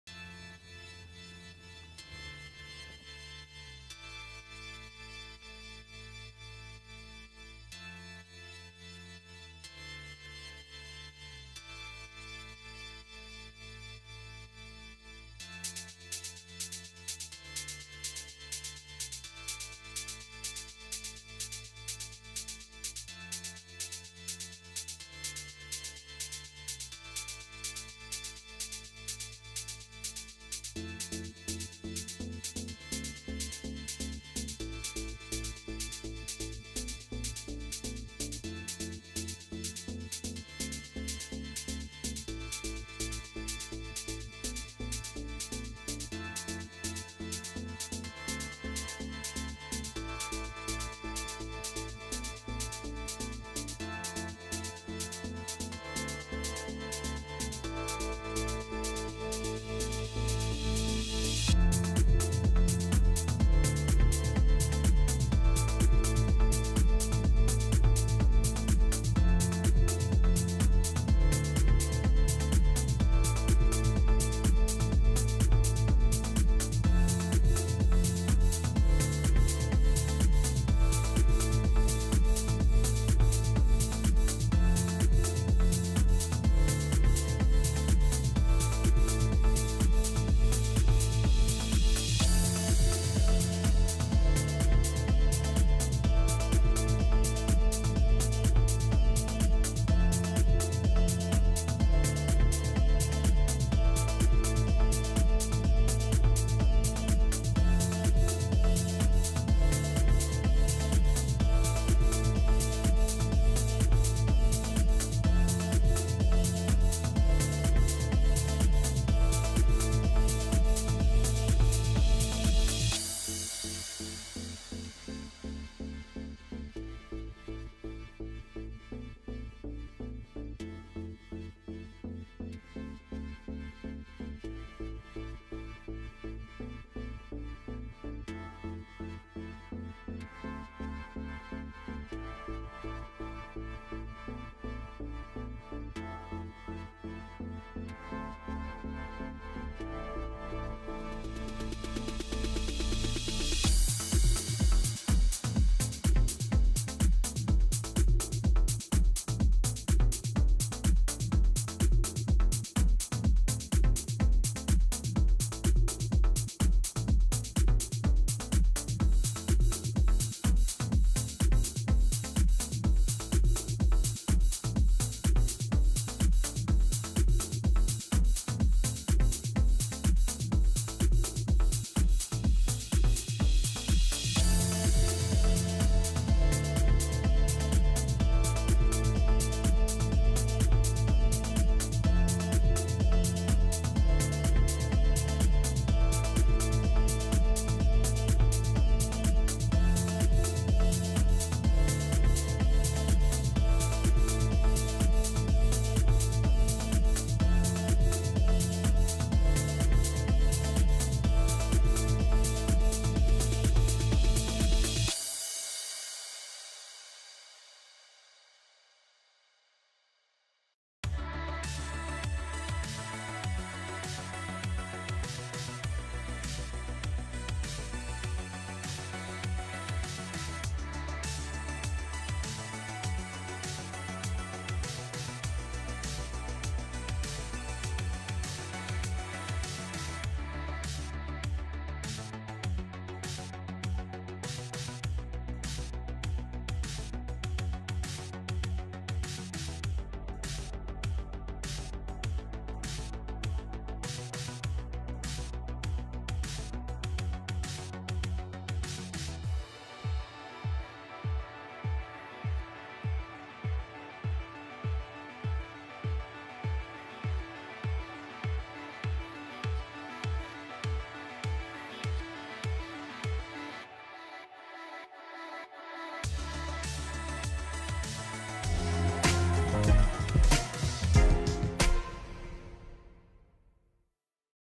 LEGO Claw Machine Tutorial!
Here is a claw machine you could put in a LEGO arcade, mall, beach, and many other builds.
Artist: Chris James